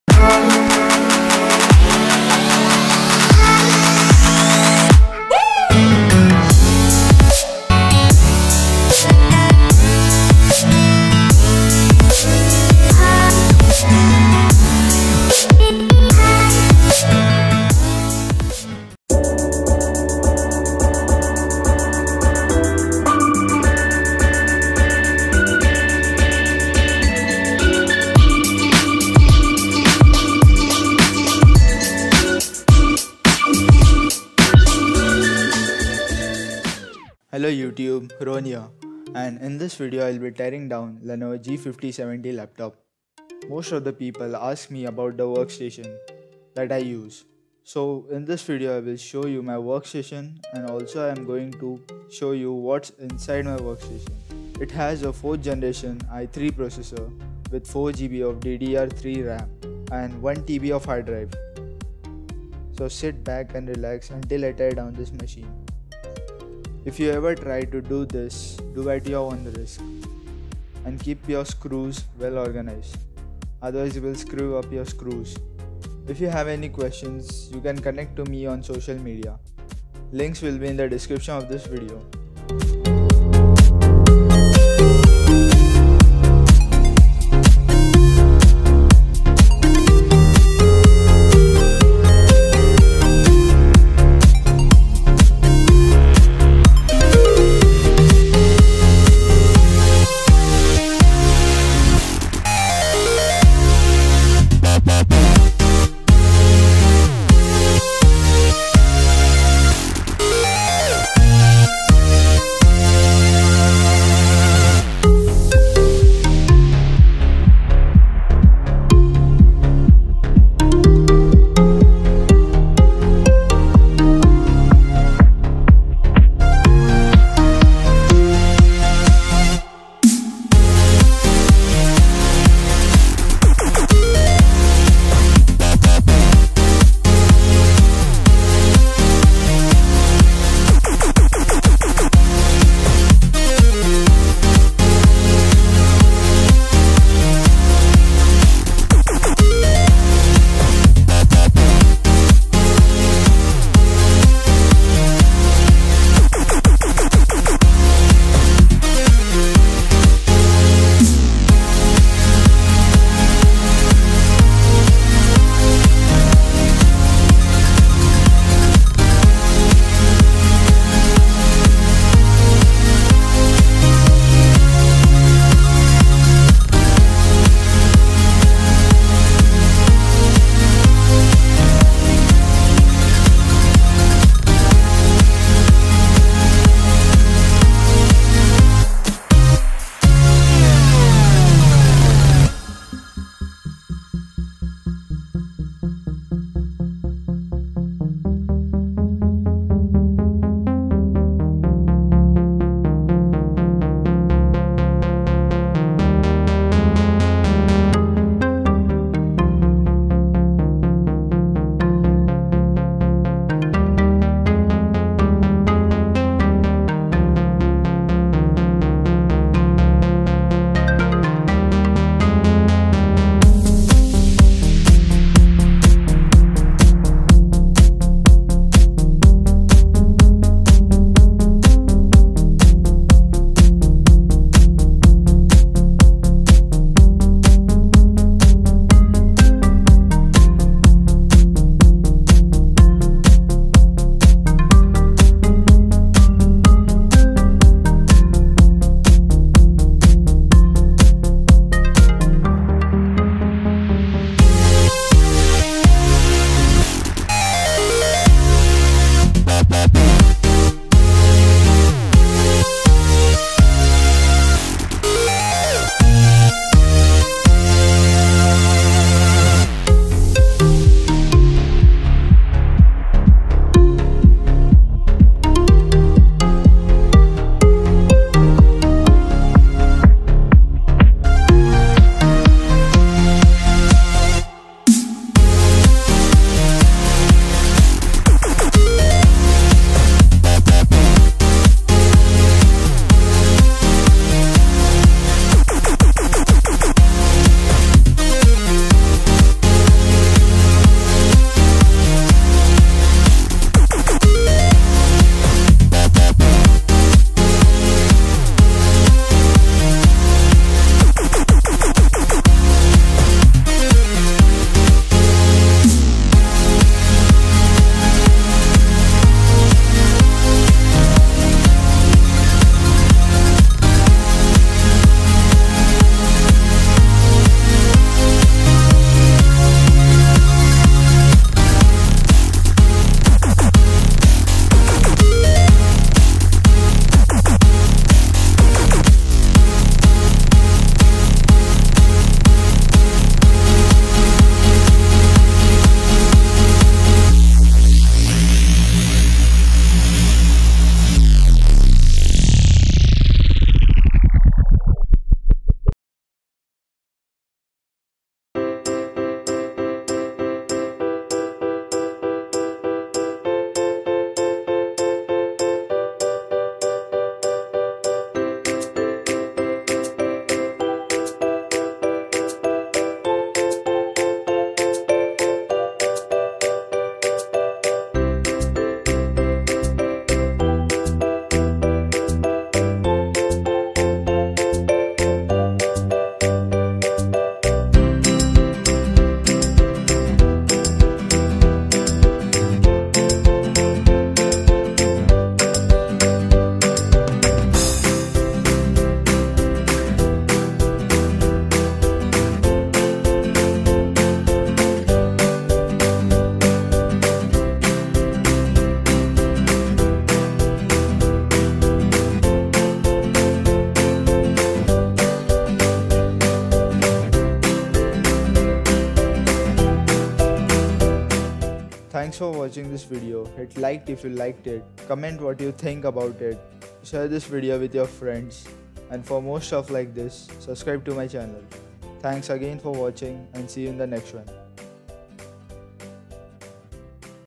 Lenovo Laptop Teardown / Repair | G50-70
In this video i will show you how to teardown a laptop.